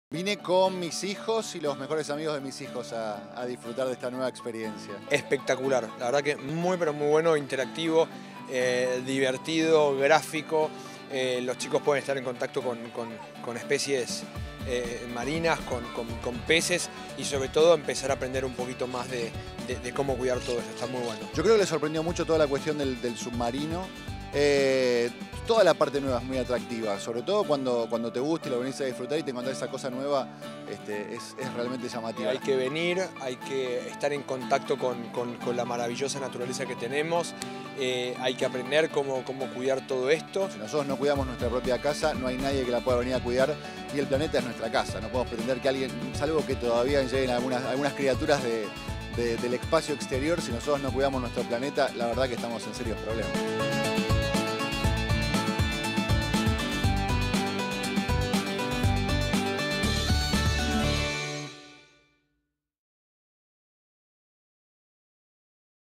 Fundación Temaikèn - Horacio Cabak e Iván de Pineda en el CEA
¡Junto a sus familias exploraron el nuevo Centro de Exploración Acuática y nos cuentan sus impresiones!